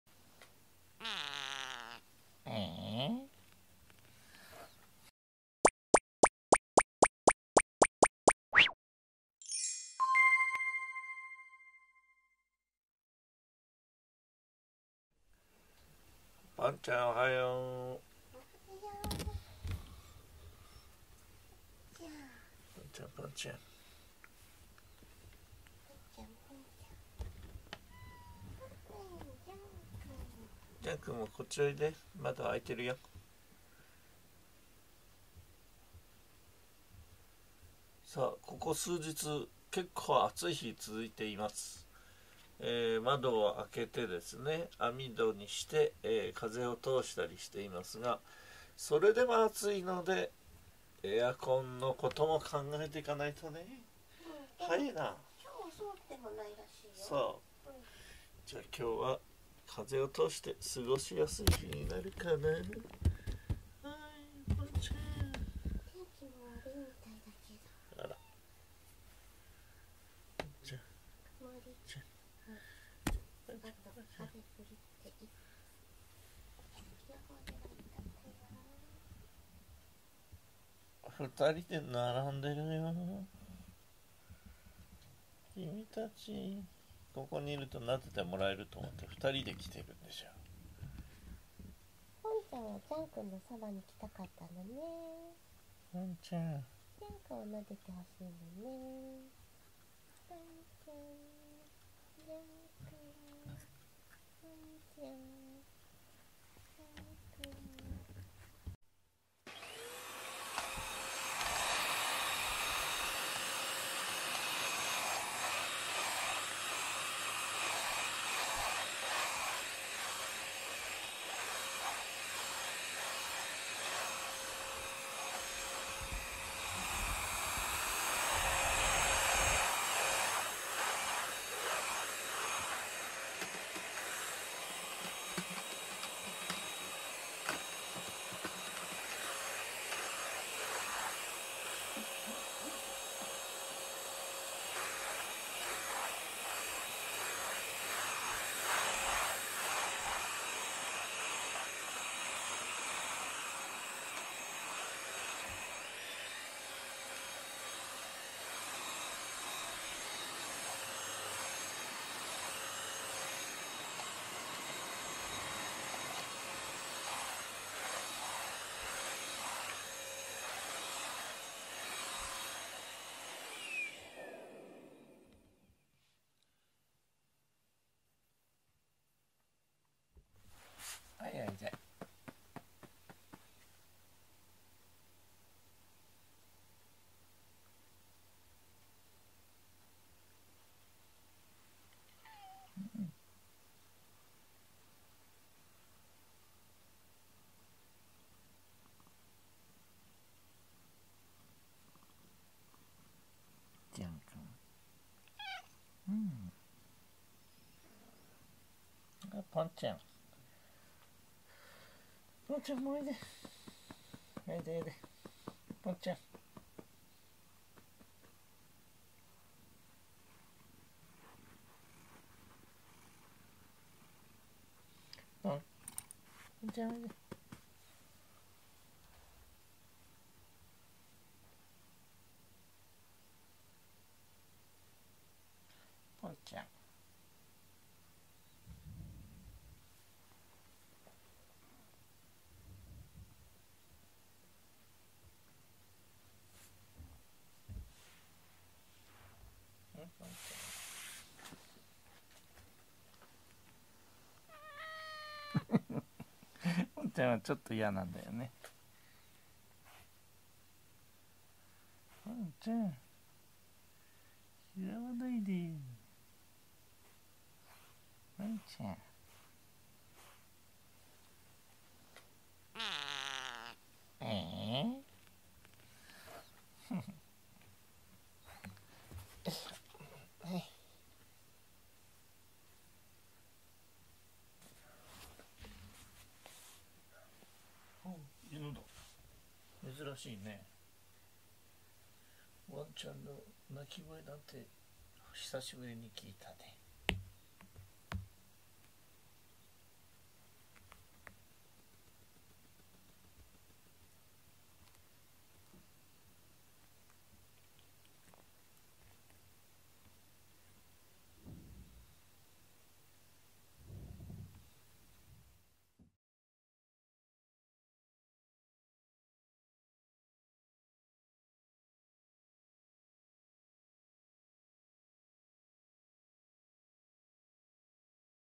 パパ！キスはまだイヤなの！！乙女な黒猫ポンちゃん【Jean & Pont 2487】2021/5/16 保護猫ジャンけんポン
ずいぶん甘えるようになってきたポンちゃんなので、パパはちょっと調子に乗って背中にチューを試みました。
逃げ出したりはしないので完全に嫌がっているわけではないのですが、なんか変な声で泣きました鳴きました。
ん？っと疑いの気持ちが起きたのでもう一回キスをしてみたらもっと変な声で鳴きました。
やっぱイヤなんだ。